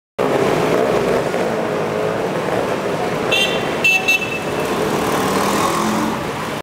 Vespa meeting in Battipaglia (Salerno) Italy at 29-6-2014
Raduno a Battipaglia il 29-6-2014
Vespa Treffen in Battipaglia, Provinz von Salerno, Italien am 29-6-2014